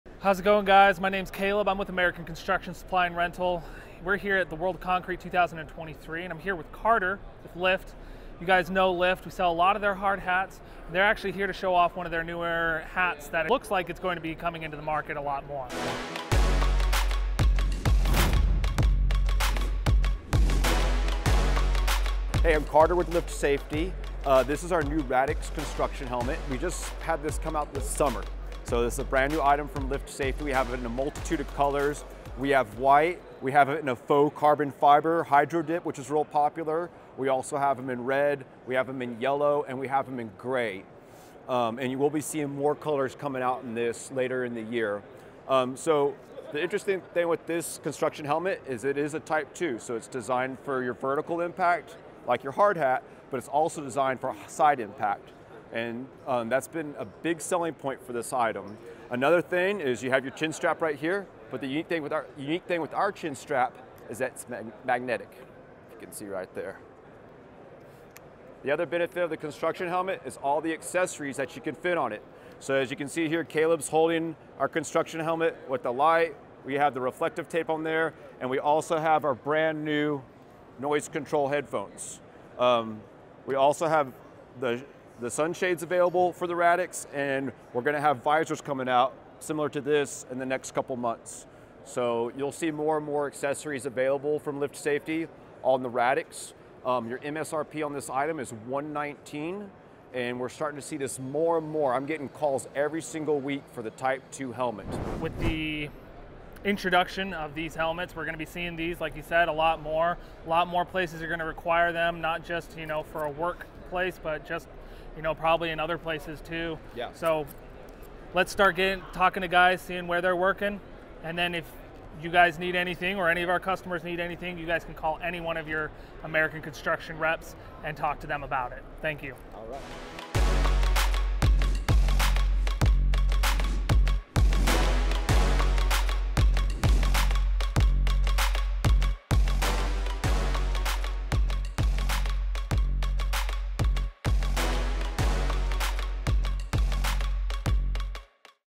Lift RADIX Safety Helmet
The RADIX Vented Safety Helmet is comfortable thanks to its four-point LUX Suspension system, which guarantees the helmet will fit securely on the head. The magnetic quick-lock, adjustable chinstrap makes it ideal for working at height and on the ground. The ventilated outer shell protects against impact hazards. With its potential for integrating an ARCLIGHT headlamp, LIFT hearing protection, and multiple accessories, it is an entirely modular helmet, thus meeting the specific needs of multiple industries.

CERTIFICATIONS: ANSI Z89.1, TYPE 2, CLASS C, Certified ANSI Type 2 standards, protects from side, front and rear impacts